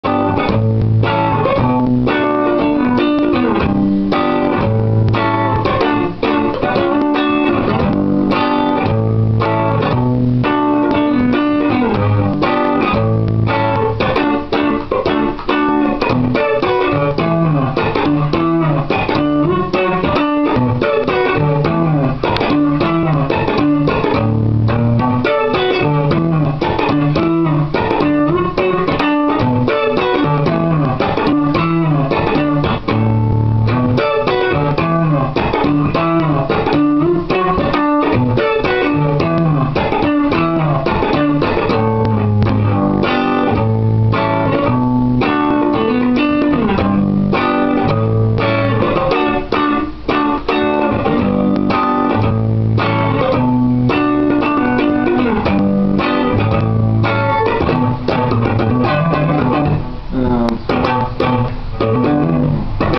Funky-Mania 2011 Ruslan. Guitar college Moscow. Professor K.Kremnev
Guitar College Moscow, funk-guitar classes, prof. Konstantin Kremnev, advanced level.
Ruslan Nikitin plays his hot riff.
Гитарный колледж, класс Константина Кремнёва, курс "фанк-гитара", старшая группа.
Руслан Никитин исполняет свой новый крутой рифф.